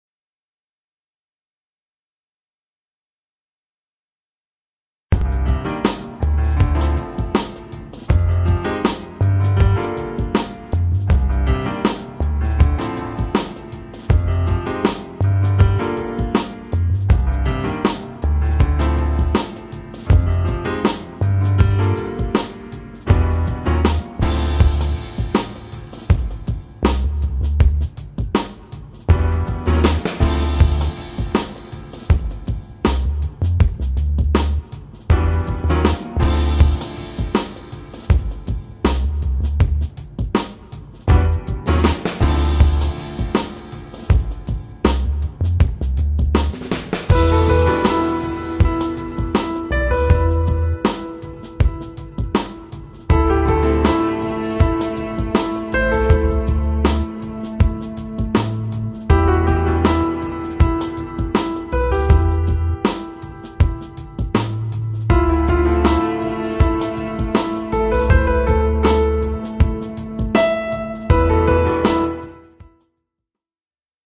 Renault zoe occasion visible à Albi présentée par Automobiles albigeoises
Cette Renault zoe occasion disponible à Albi dans le 81 est mise en vente par Automobiles albigeoises - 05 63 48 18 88 - tous les détails sont